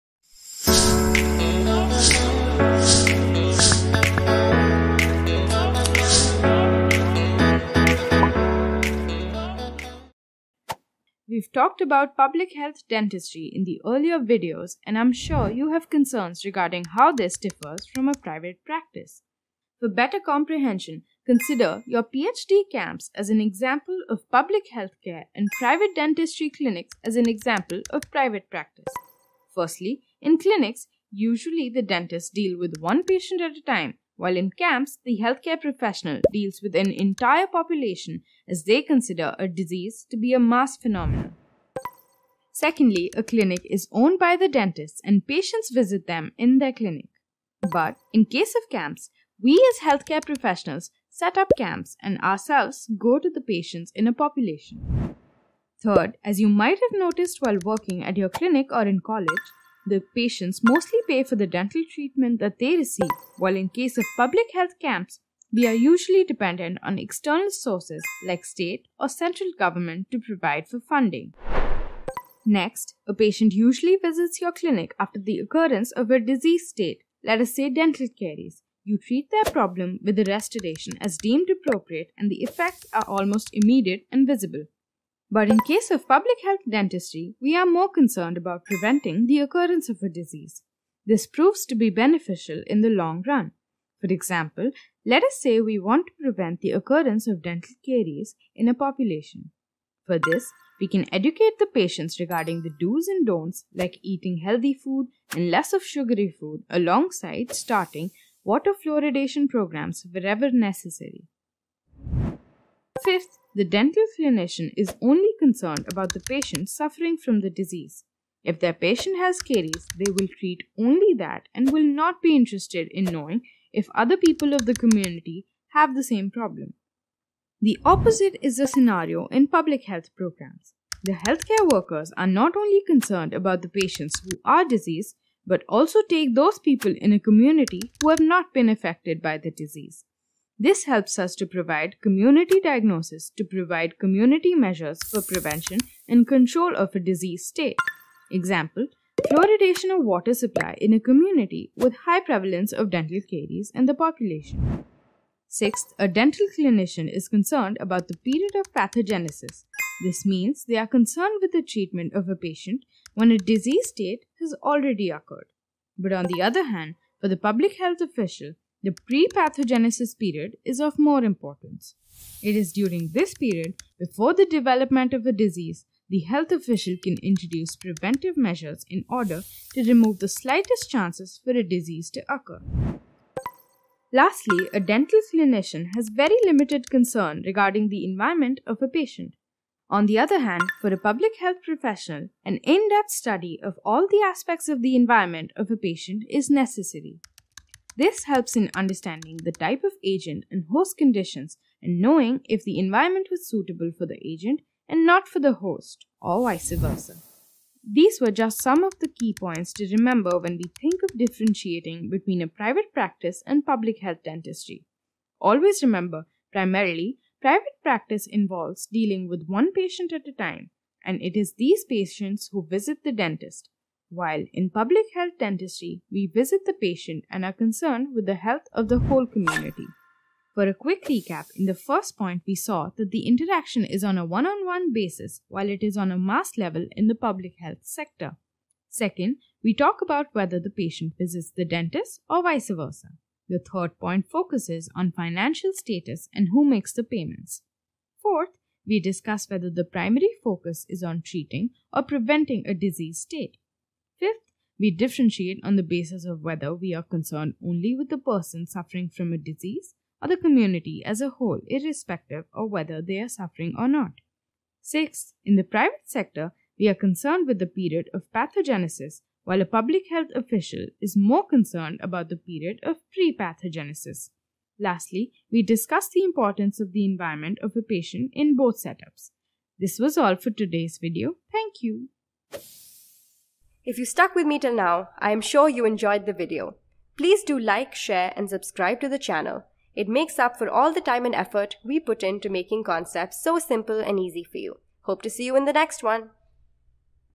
Public Health Dentistry | Funding, Community Diagnosis & Key Practices
Welcome to our enlightening video on **Introduction to Public Health Dentistry**! In this must-watch episode, we dive deep into essential **funding sources for Public Health Camps** that provide crucial dental care to underserved communities. Join us as we explore innovative strategies used for **community diagnosis in Public Health**, revealing how these practices can improve oral health outcomes for all. 🦷 Finally, we highlight the **key differences between practices** in public health and private dentistry. This engaging discussion is packed with valuable insights for students, professionals, and anyone passionate about improving public health.

Time stamps:
0:11 - Introduction to Public Health Dentistry
1:13 - Funding sources for Public Health Camps
2:09 - Community diagnosis in Public Health
3:34 - Key differences between practices